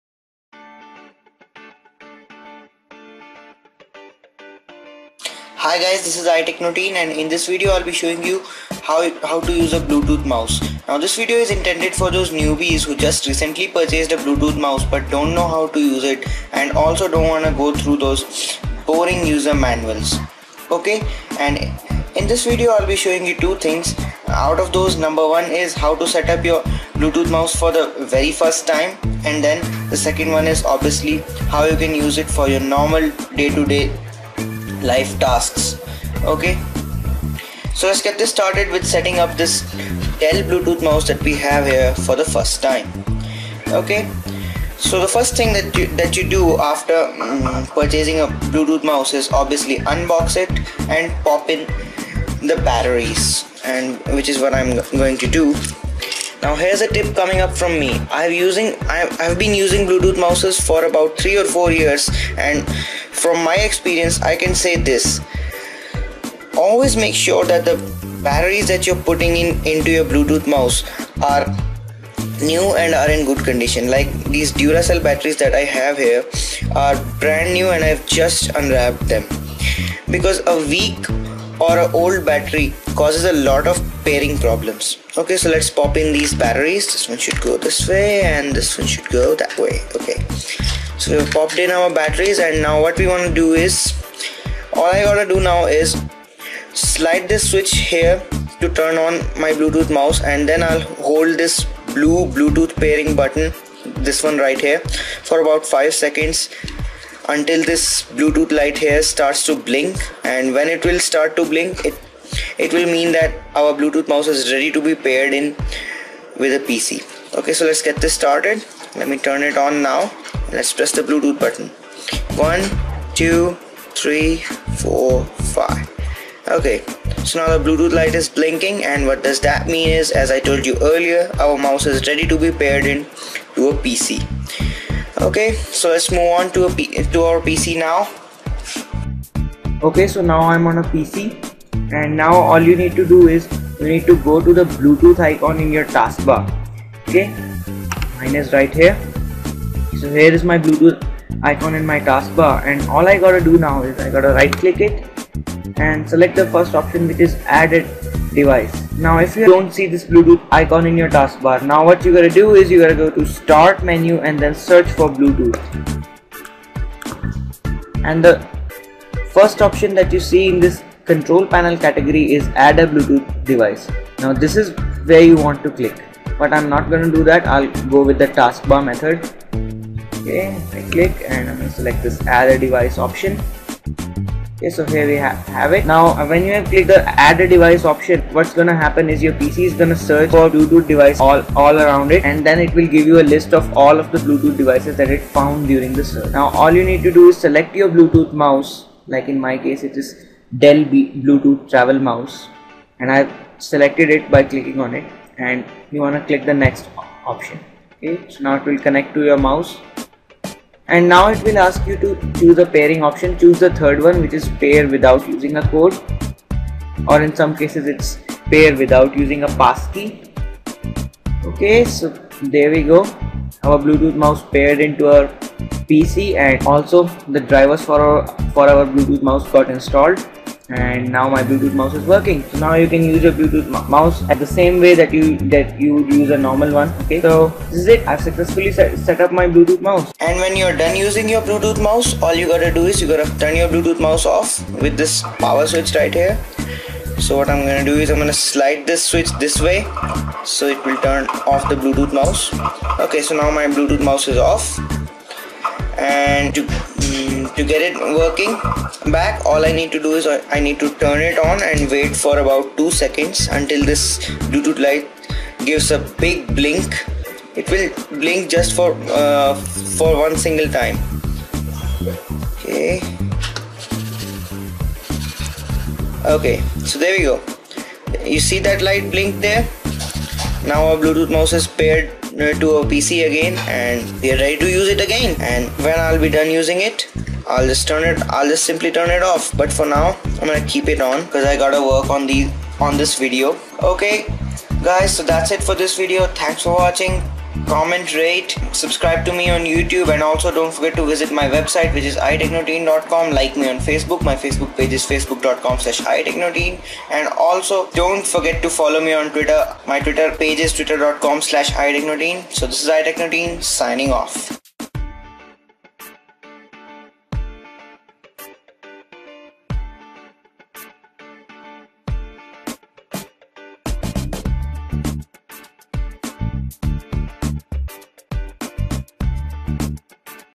How to set up and use a bluetooth mouse ?
Hi guys,in this video I showed you how to use a bluetooth mouse covering two major things that every bluetooth mouse newbie must know to get started out of which number one is how to set up your newly purchased bluetooth mouse for the very first time and the second one is how to use it for your normal daily life needs.